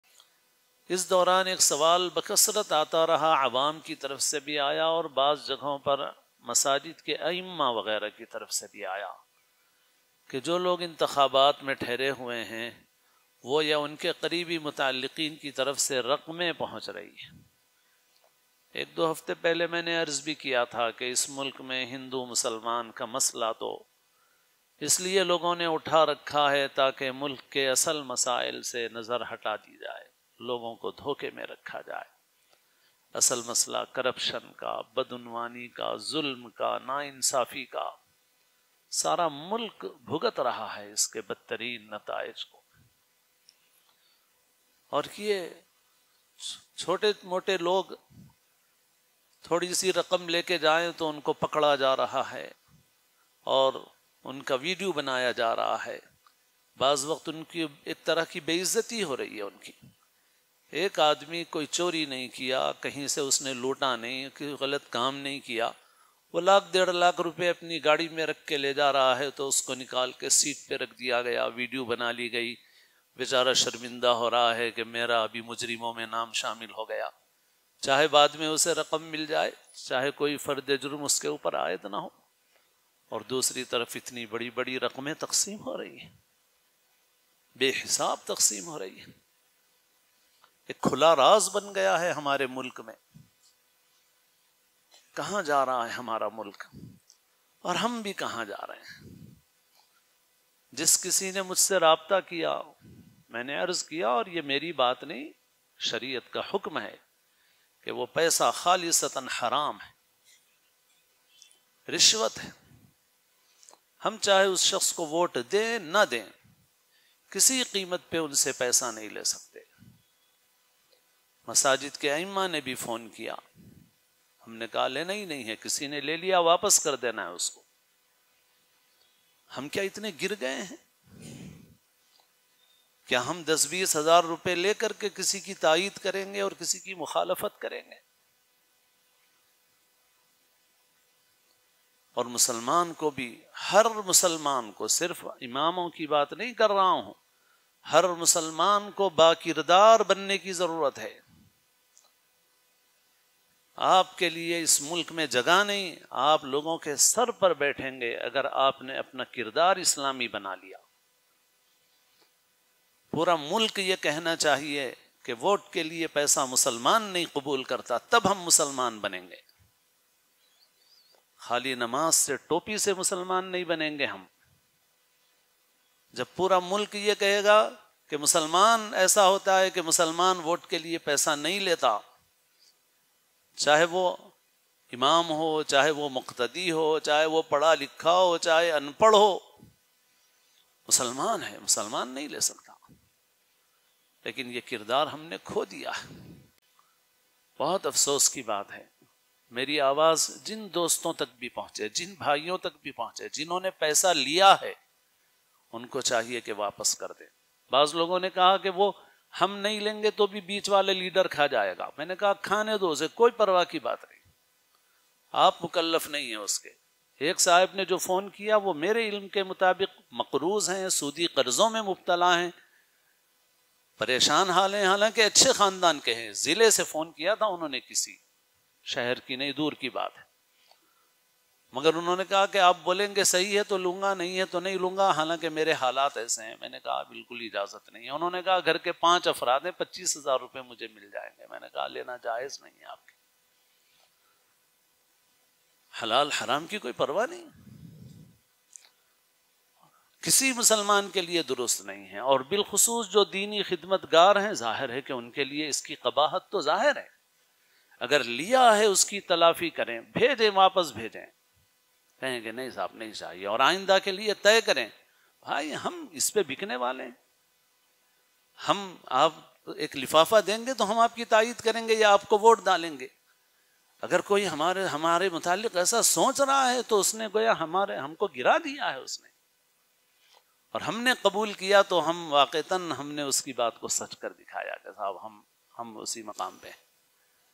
Paise k Badle Vote! || ! یسے کے بدلے ووٹ
Paise k Badle Vote! || Hazrat Maulana Obaidurrahman Ather Sahab Damat Barkatuhum

یسے کے بدلے ووٹ! || حضرتِ مولانا احمد عبیدالرحمٰن اطہر صاحب دامت برکاتہم